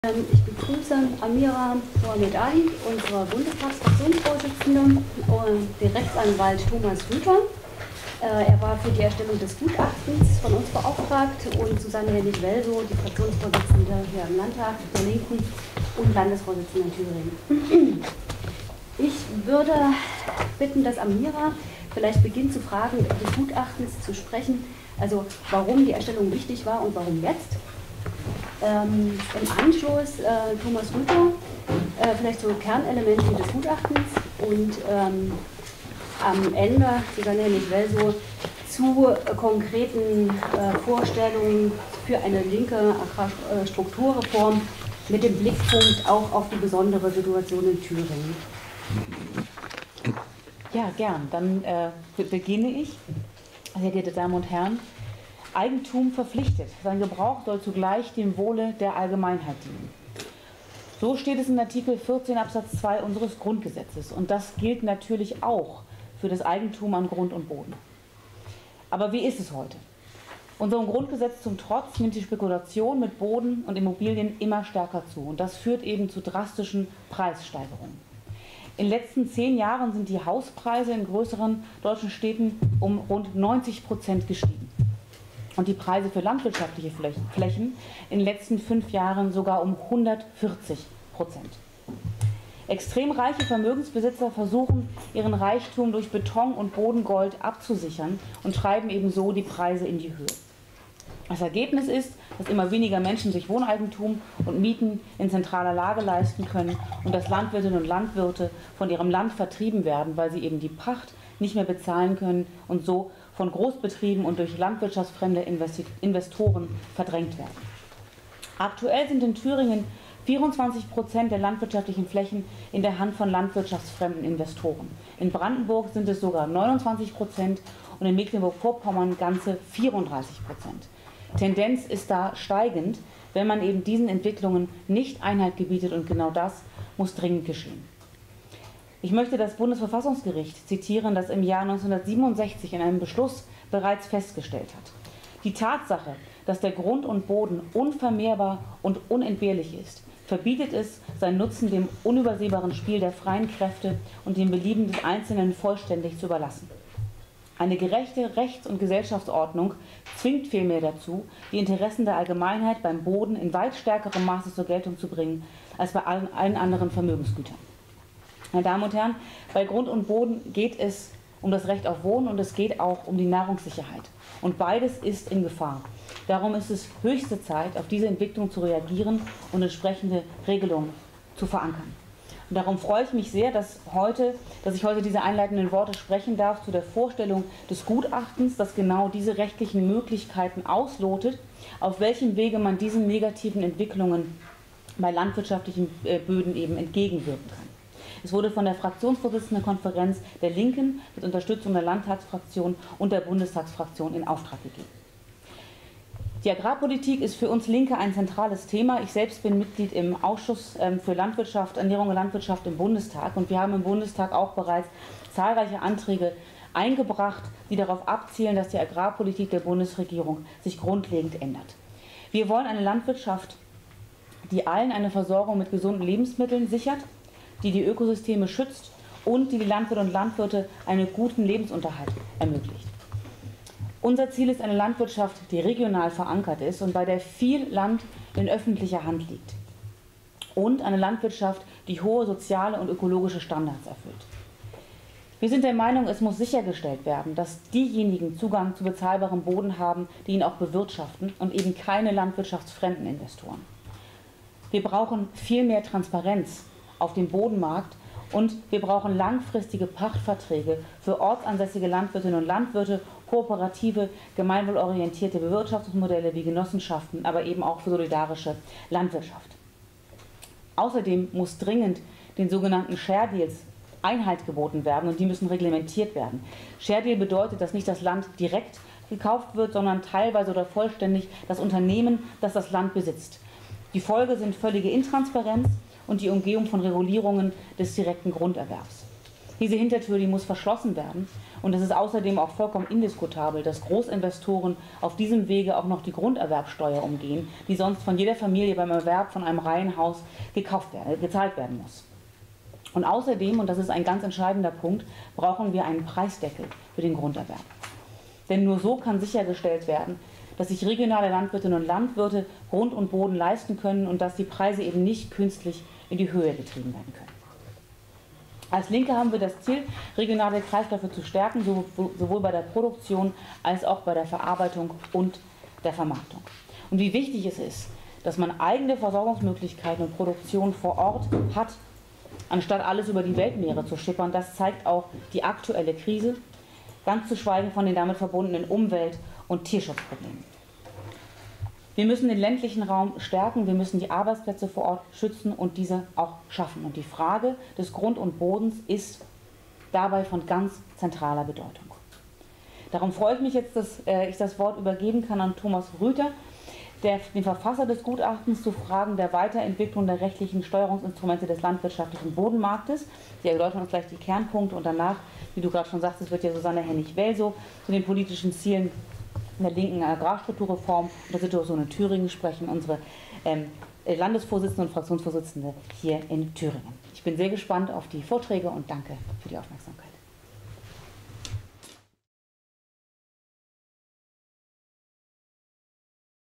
Amira Mohamed Ali: Explosion der Bodenpreise Einhalt gebieten
Die Preise für Bauland, besonders aber für Agrarland, sind in den letzten Jahrzehnten explosionsartig gestiegen. Normalverdienende können sich kaum noch das Wohnen in zentralen Lagen leisten, und Landwirt*innen werden von ihrem Ackerland verdrängt, weil sie die Pacht nicht mehr erwirtschaften können. Investoren, die auf Rendite aus sind, heizen die Entwicklung weiter an. Unsere Fraktionsvorsitzende Amira Mohamed Ali  hat am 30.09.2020 in Erfurt gemeinsam mit Susanne Henning-Wellsow,  Vorsitzende der Fraktion DIE LINKE im Thüringer Landtag, ein Gutachten vorgestellt, das die rechtlichen Rahmenbedingungen auslotet, mithilfe derer die Bodenpreisspirale und der Vormarsch landwirtschaftsfremder Investoren gestoppt werden können.
Weitere Informationen und das Gutachten als PDF zum Herunterladen finden Sie